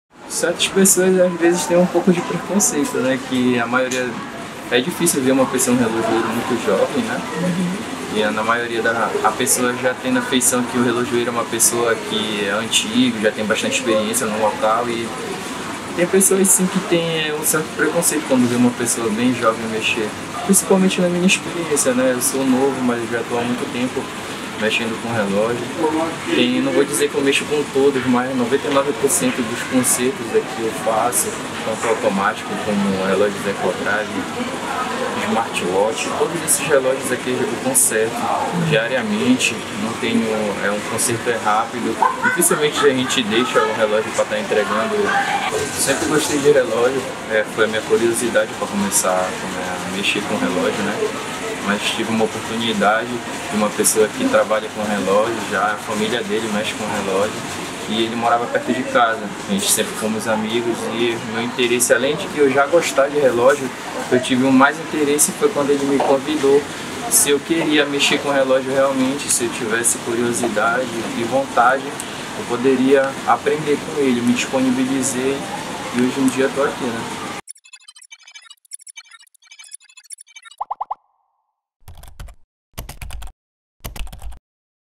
Jovens apostam em profissões consideradas “antiquadas”
Apesar da maioria dos jovens, hoje, preferir carreiras mais afinadas com a tecnologia e internet, ainda há uma parcela que se identifica com profissões consideradas extintas ou em processo de extinção